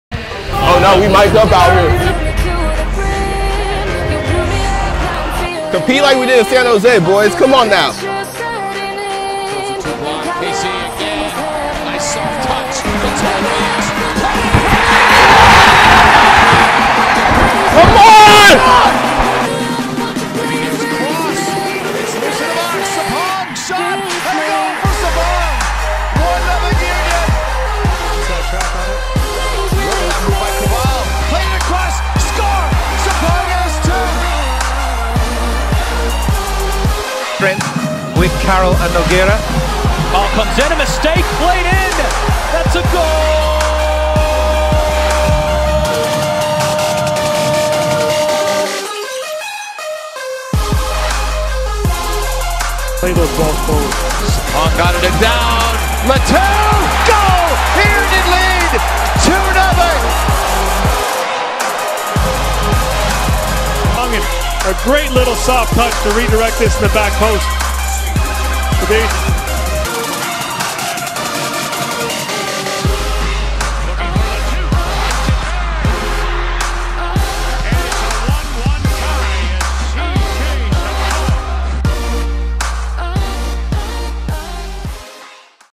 Cj Sapong Goals And Celebration
Charles "C. J." Nana Kwabena Sapong is an American soccer player who currently plays for Philadelphia Union in Major League Soccer, highlights.